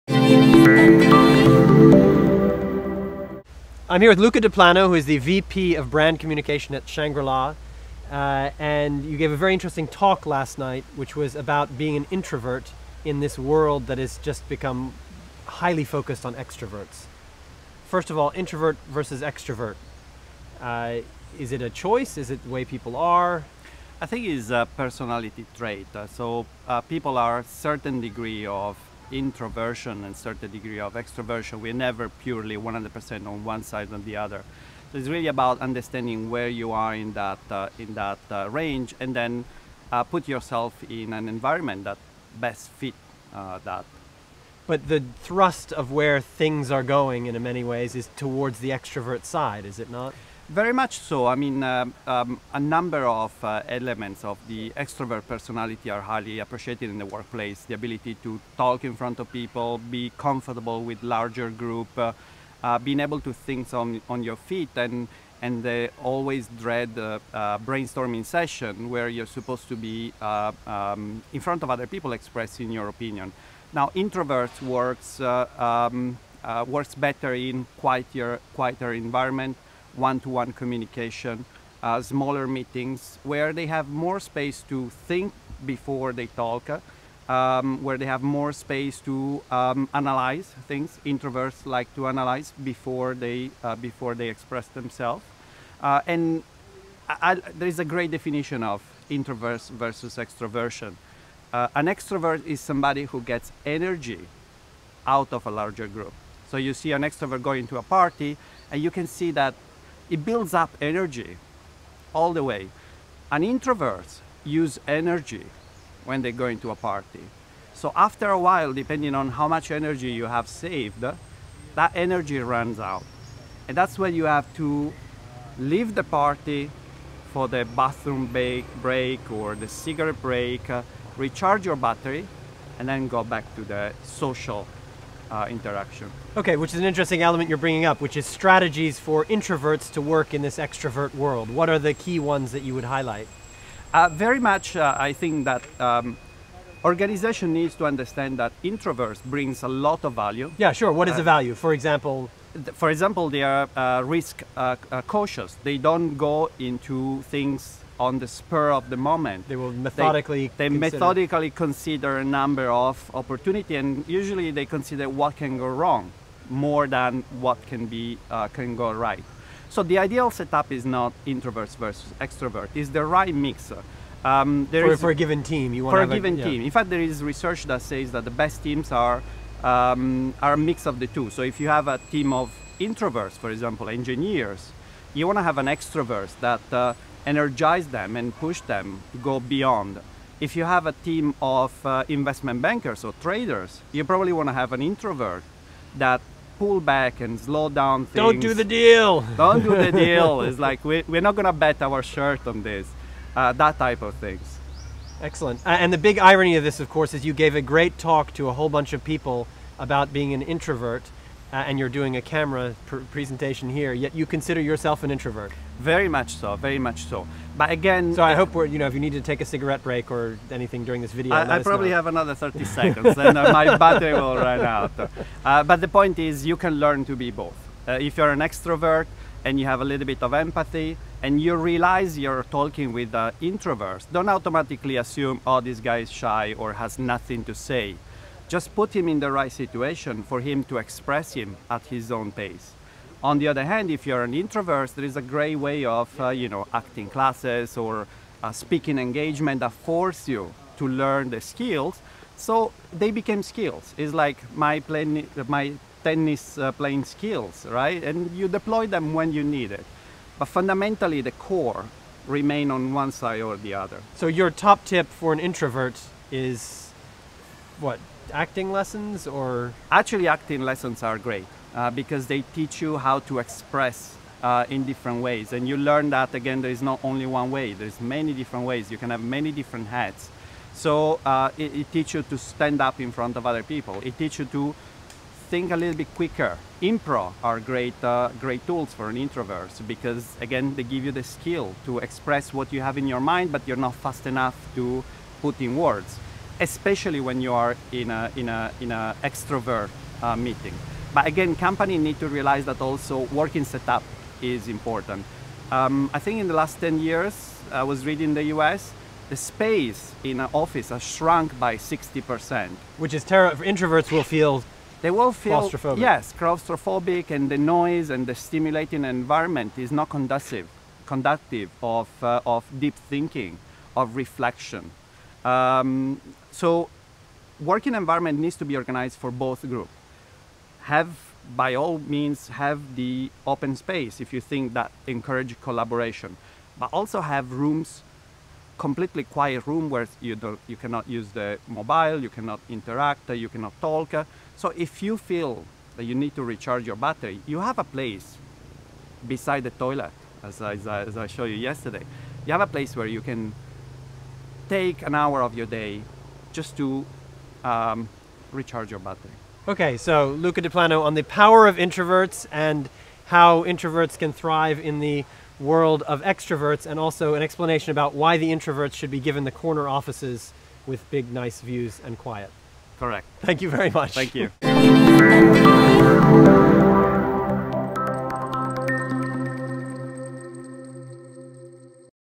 WPP Stream Asia: Thomas Crampton and Luca Deplano
ogilvydo was at WPP Stream Asia 2014 in Langkawi. Here Luca Deplano, VP of Shangri-La Hotels APAC, shares his fascinating views on being an introvert in a world focussed on extroverts, and why organisations need both types.